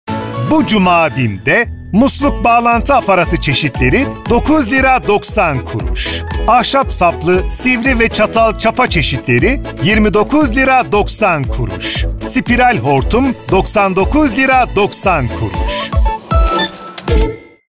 25 Mart Aktüel - Musluk Bağlantı Aparatı Çeşitleri, Ahşap Saplı Çapa, Spiral Hortum
Bu Cuma BİM'de Musluk Bağlantı Aparatı Çeşitleri 9,90 TL, Ahşap Saplı Çapa 29,90 TL, Spiral Hortum 99,90 TL.